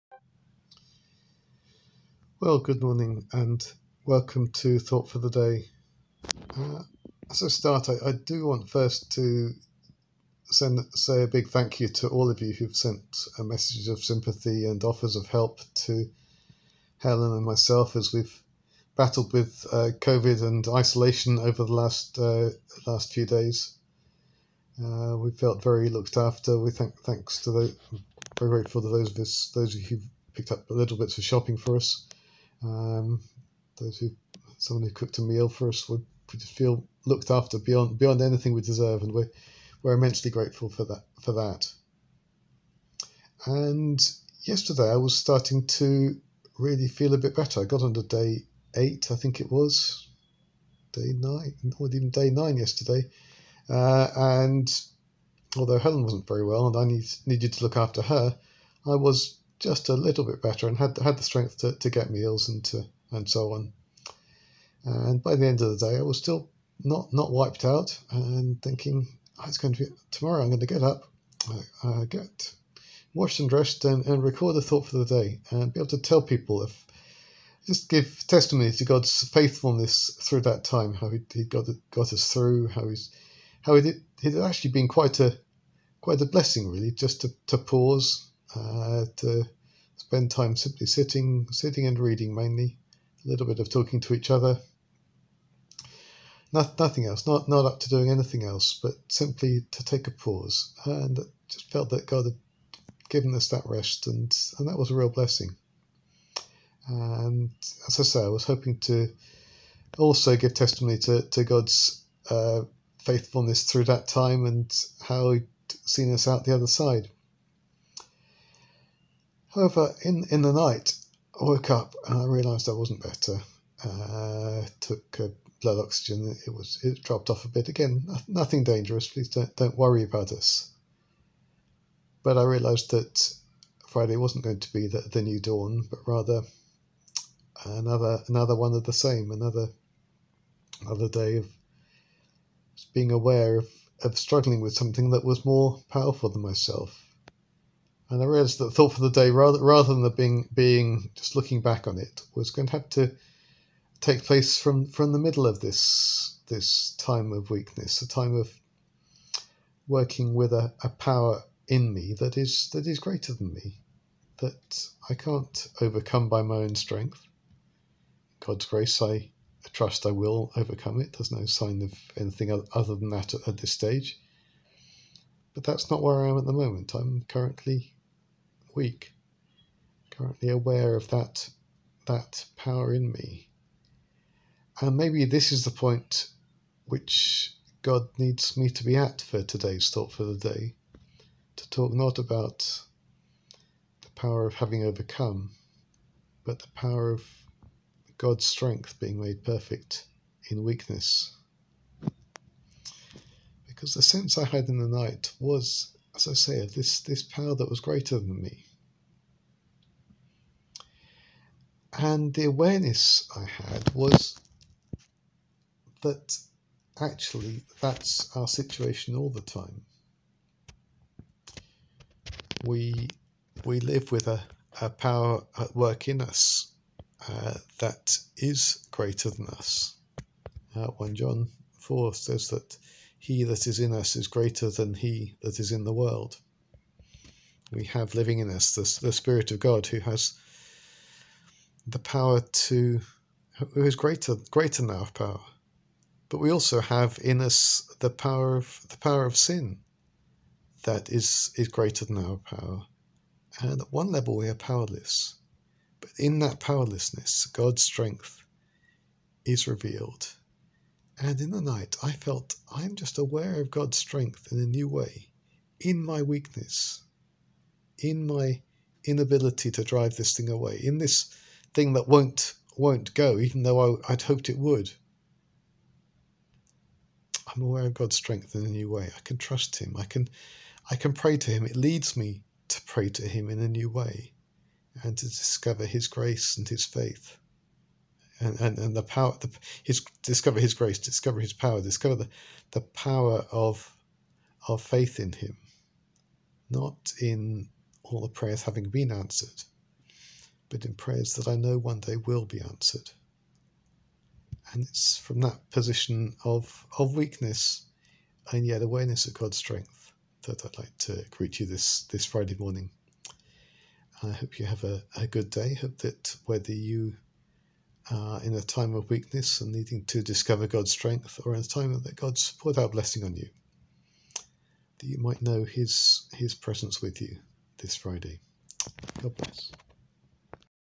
Thought for the day - A greater power
Tom shares his experience of trusting God in the middle of Covid